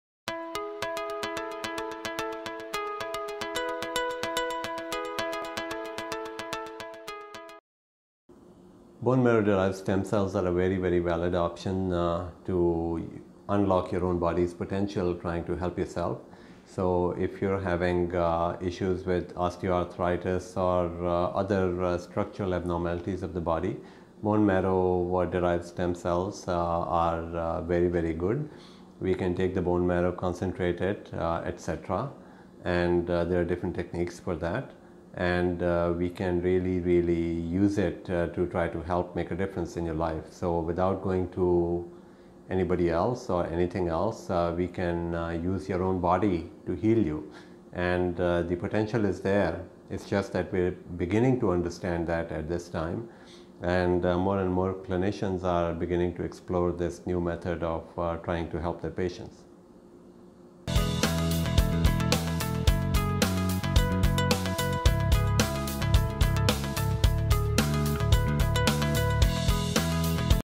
Bone Marrow Stem Cells
Bone Marrow Mesenchymal Stem Cells at American Stem Cell Institute